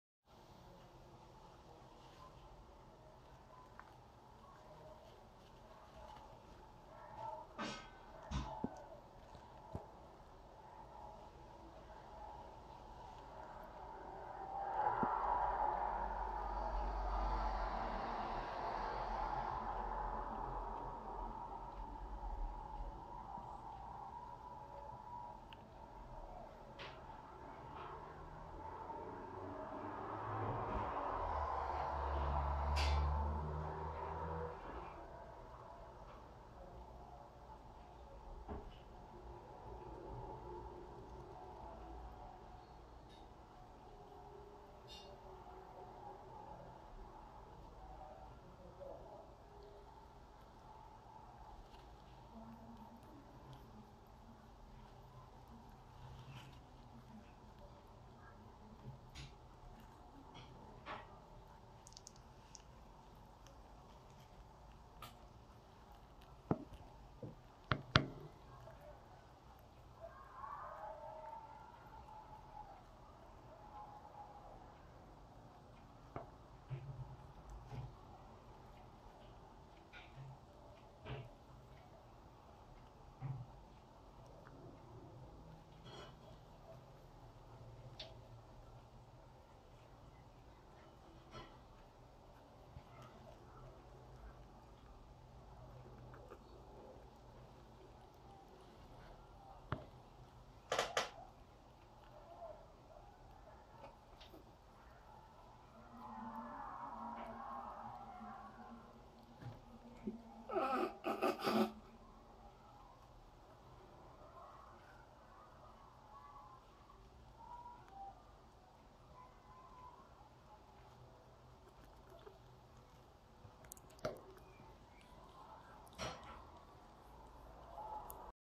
Chibi & Shinjiro
Shinjiro myje bráchu Chibiho a snaží se umýt si ucho.